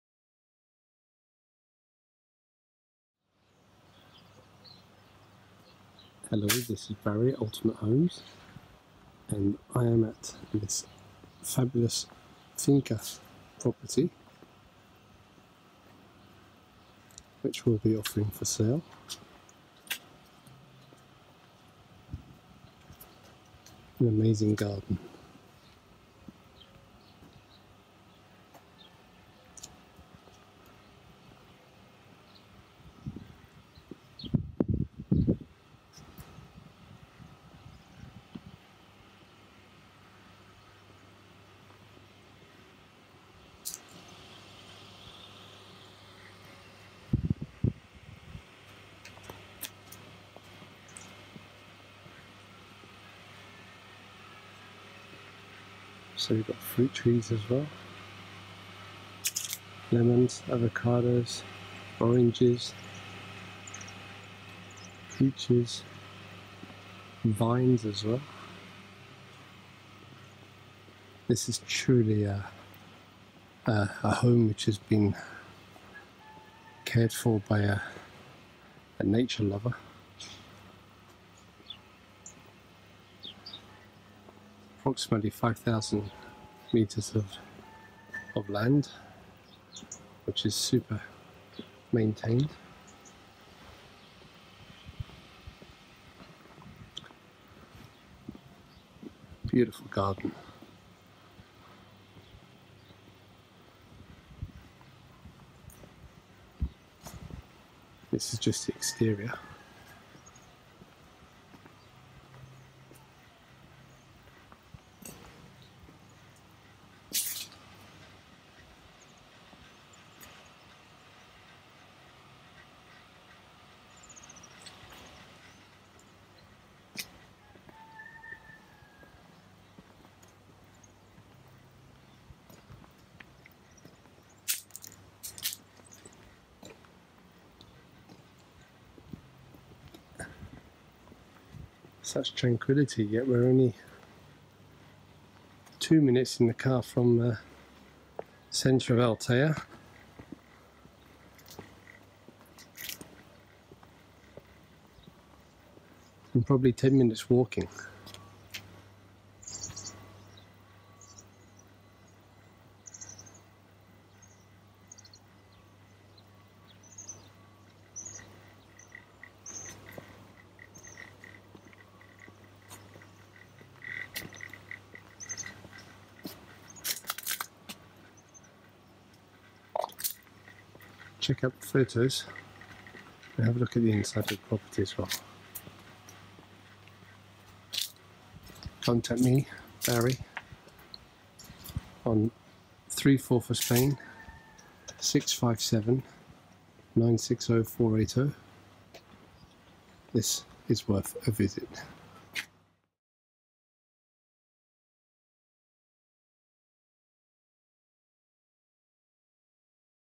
SOLD - Altea Quintana
Fabulous country home offered for sale in Altea (Alicante), with five bedrooms in total distributed over two floors, each with independent living facilities and their own private entrance.
Superb location being only minutes from the town and the beach.